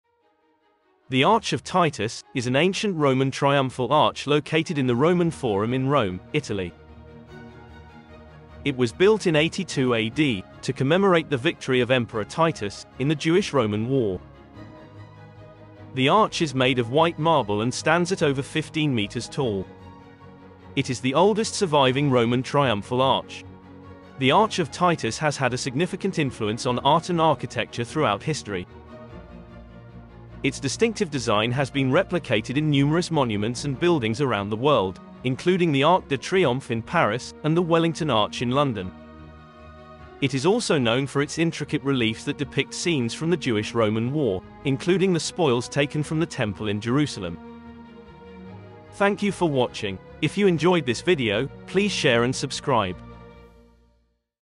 Discovering the Oldest Surviving Triumphal Arch: the Arch of Titus in Rome
The Arch of Titus is an ancient Roman triumphal arch located in the Roman Forum in Rome. It was built in 82 AD to commemorate the victory of Emperor Titus in the Jewish-Roman war. The arch is made of white marble and stands at over 15 meters tall.

One interesting fact about the Arch of Titus is that it is the oldest surviving Roman triumphal arch. It is also known for its intricate reliefs that depict scenes from the Jewish-Roman war, including the spoils taken from the Temple in Jerusalem.

Another fun fact is that the Arch of Titus has had a significant influence on art and architecture throughout history. Its distinctive design has been replicated in numerous monuments and buildings around the world, including the Arc de Triomphe in Paris and the Wellington Arch in London.

Today, the Arch of Titus is a popular tourist attraction and a symbol of ancient Roman power and triumph. Visitors can admire its intricate carvings and learn about the history and significance of this impressive monument.